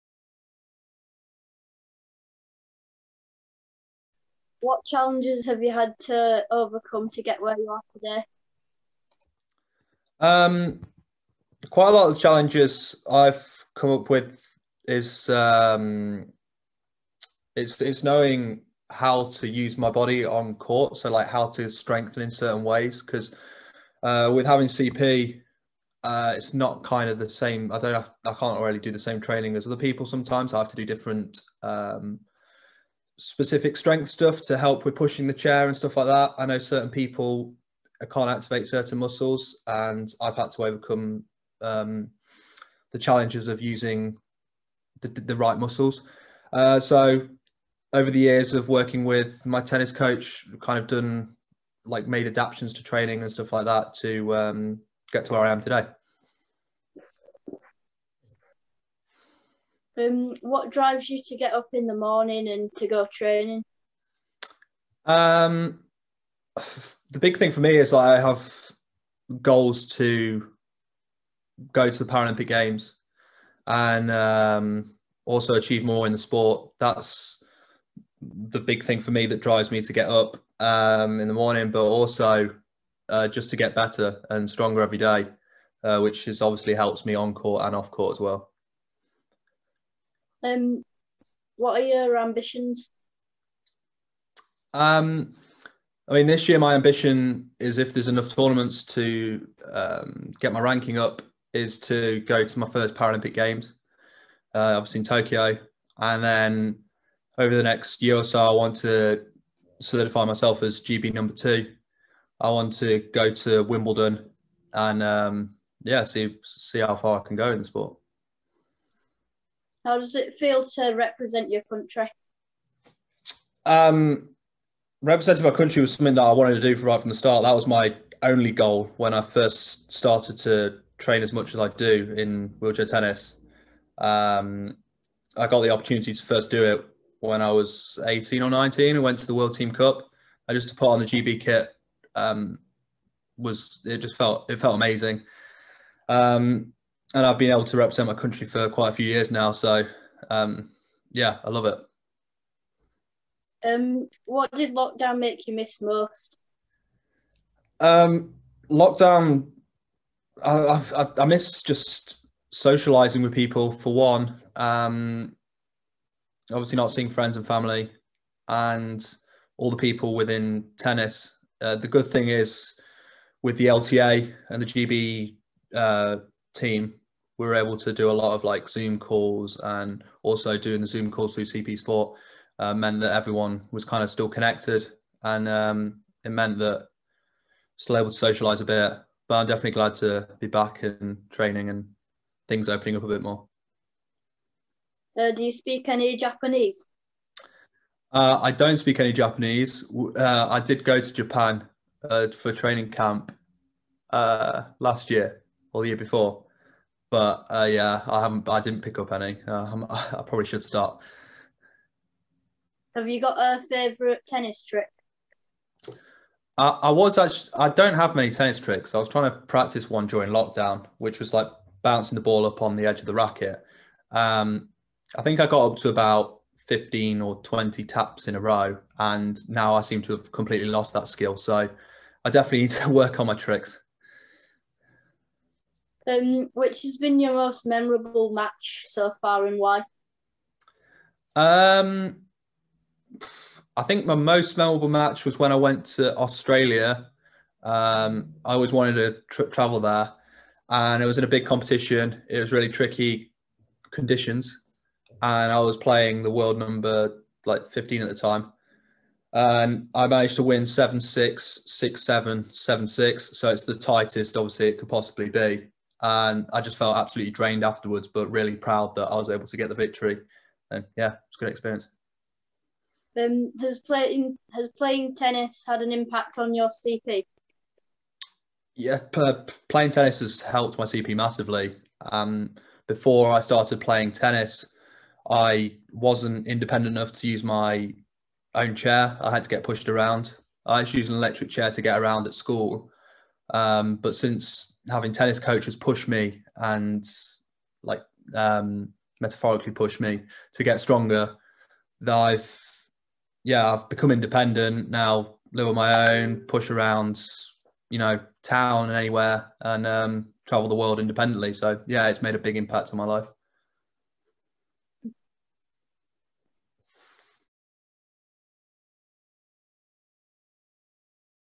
Lucas interviews Wheelchair Tennis player James Shaw
Young Wheelchair Tennis player and CP Sport member Lucas interviews 6 x National Champion and CP Sport Ambassador James Shaw to discuss lockdown, ambitions, and the impact of playing tennis.